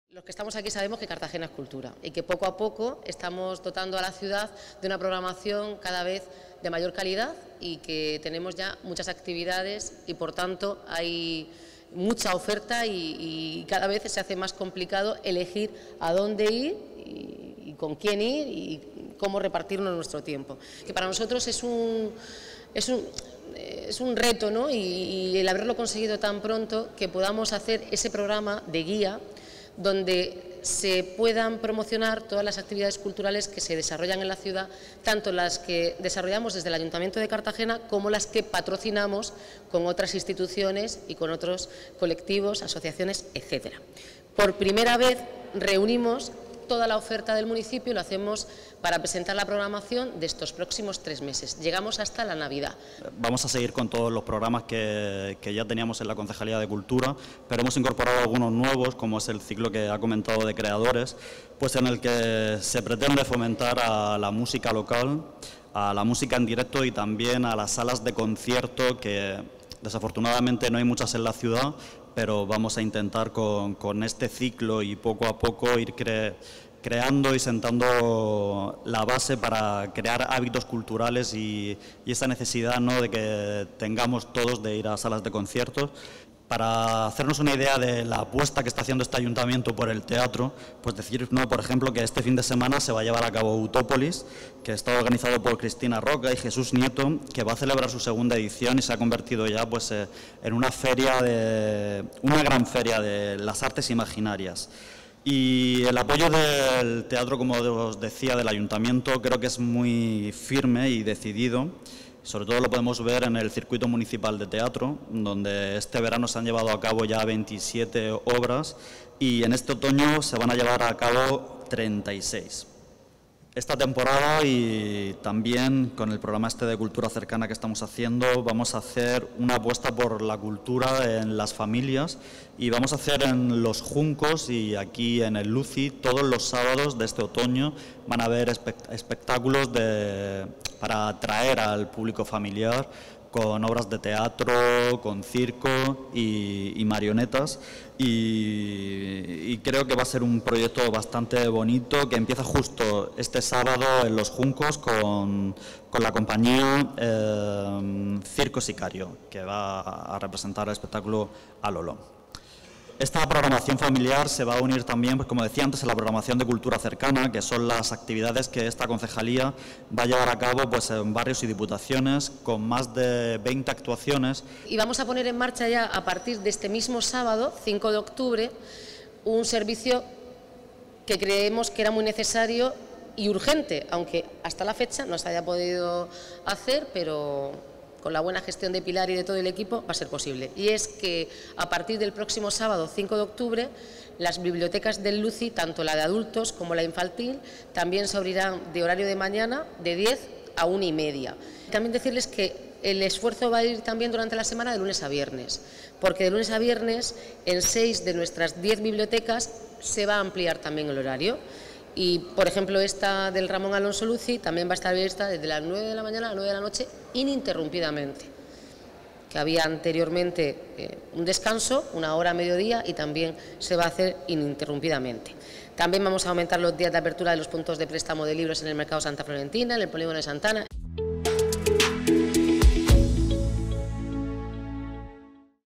Pedro Simón, Ara Malikian, Anabel Alonso, Pat Metheny, Salvador Sobral, Sabina Urraca, María León...
Más de 150 actividades culturales se llevarán a cabo en Cartagena, este otoño. Nombres como Santiago Posteguillo, Pedro Simón, Lori Meyers, Luis Piedrahita, Ara Malikian, Eva Hache, Anabel Alonso, La Carroza del Teatro Real, Goyo Jiménez, Pat Metheny, Salvador Sobral, Sabina Urraca, María León, Juan Ramón Lucas, Berto Romero, Cristina García Rodero, Silvia Abril o El Kanka pasarán este otoño por Cartagena, según ha anunciado la alcaldesa de Cartagena, Noelia Arroyo, en un acto, junto al sector cultural de la ciudad.

"Estamos aquí para presentar la programación cultural de otoño de Cartagena. No digo la programación del Ayuntamiento de Cultura, digo la programación de otoño de la ciudad, porque hoy damos el pistoletazo de salida a un gran cantidad de actos y eventos culturales, que organizamos entre todos, Ayuntamiento y demás instituciones, asociaciones y entidades que nos acompañéis" ha señalado la alcaldesa en el acto.

"Por primera vez en Cartagena se ha hecho una agenda cultural en papel que reúne en un solo librillo, toda la programación de nuestra ciudad. Una de mi apuestas con la creación de la dirección general de cultura era la de la coordinación no solo de los eventos, si no de toda la agenda de ciudad, y como resultado, este programa que va a ser una genial guía para que los ciudadanos sepan qué actividad cultural puede hacer cada día. Desde esta semana a Navidad" ha comentado Arroyo.

BIBLIOTECA, ABIERTA LOS SÁBADOS

"Era un servicio muy demandado por los usuarios y por fin lo vamos a llevar a cabo. Y es que a partir de este sábado, 5 de octubre, las bibliotecas del Luzzy, tanto la de adultos, como la infantil se abrirán en horarios de mañana. De 10 a 13:30 horas. Además, de lunes a viernes, en 6 de nuestras 10 bibliotecas ampliamos horario, por ejemplo esta del Luzzy va a estar abierta desde las nueve de la mañana a la nueve de la noche, ininterrumpidamente. También vamos a aumentar los días de apertura de los puntos de préstamo del Mercado Santa Florentina y el Polígono de Santa Ana" añadió la alcaldesa.

CREADORES Y MÚSICA EN VELA, NUEVOS PROGRAMAS

Esta temporada se va a llevar a cabo un nuevo ciclo musical, que se denomina Creadores. En el que se pretende fomentar al mismo tiempo a los músicos locales de Cartagena y las salas de concierto. Por eso, todos los miércoles de este otoño habrá un concierto en Mr. Witt Café. El ciclo se inaugura este miércoles, 2 de octubre, con Ari Brau, que actúo en la presentación del ciclo. Por él pasarán también Pedriñanes 77, Santiago Campillo, Valters Band, Fonoporta y Karlan. Este ciclo incluye además los conciertos que se llevarán a cabo en el Cartagena Jazz Festival de José Antonio Aarnoutse Cuarteto, Five For Jazz y Raúl Pérez Quartet.

También es novedad Música en vela. Conciertos que esta temporada se inician con alumnos y profesores del Conservatorio de Música en Cartagena, donde la música clásica se interpretará, bajo la luz tenue de cientos de velas. Estos conciertos se llevarán a acabo en el Centro Cultural Ramón Alonso Luzzy...